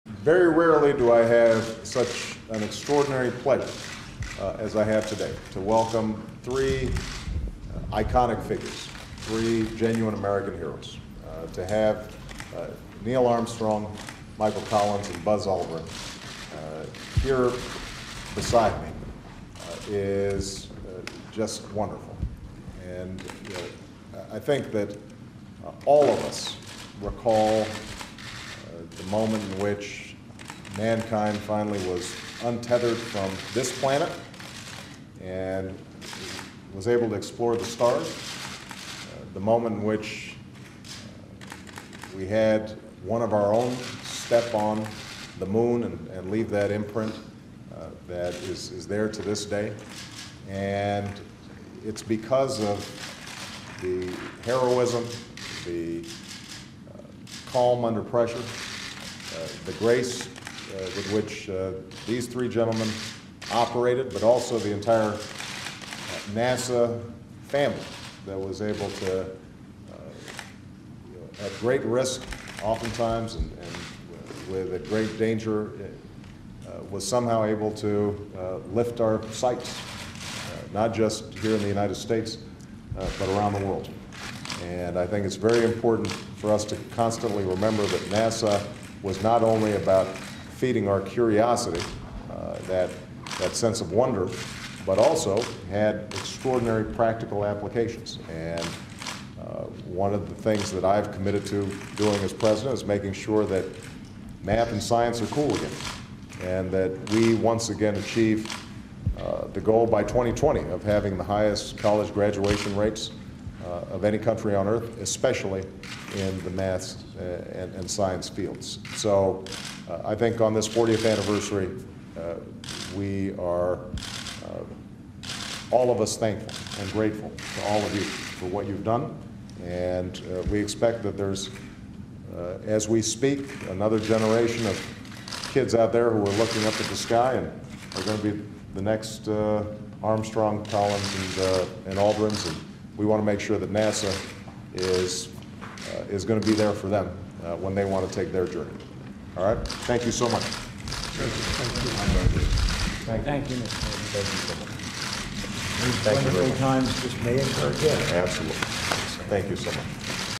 Obama Honors First Men to Land on Moon
Hailing the Apollo 11 astronauts as 'three American heroes,' President Barack Obama says exploration spurs ingenuity and inspires students in math and science. (July 20)